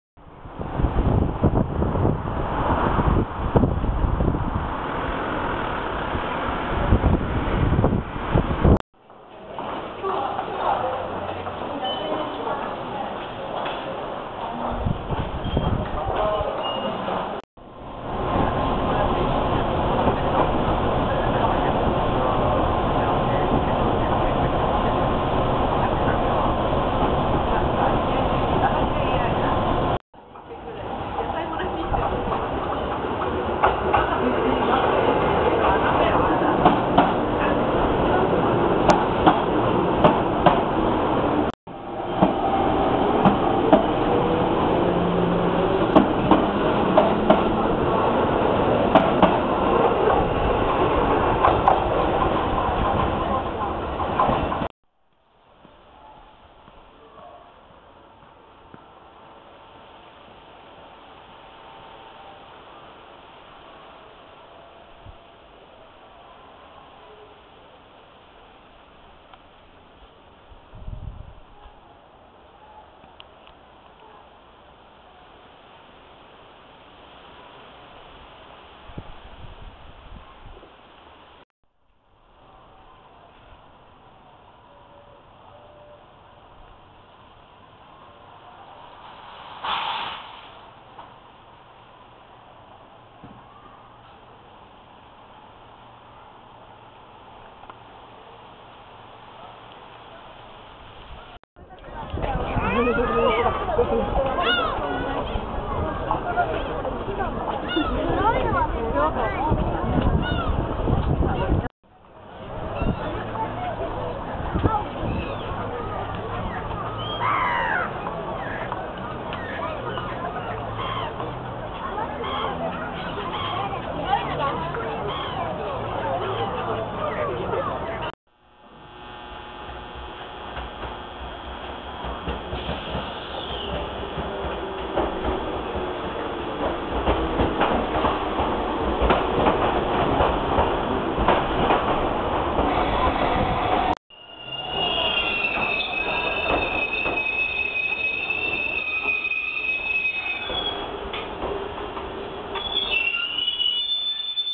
寒川・相模線・厚木駅・遊歩道猫・不忍池・江ノ電
撮影年覚えていない（ファイル移動とかすると撮影年月日が消えてしまう　怒）。たぶん2008年ごろ？
猫ちゃんは、東林間～大和市の間にある遊歩道で見た。江ノ電の駅、地元民ではなかったので、どこ駅か思い出せない。江ノ島駅かなぁ。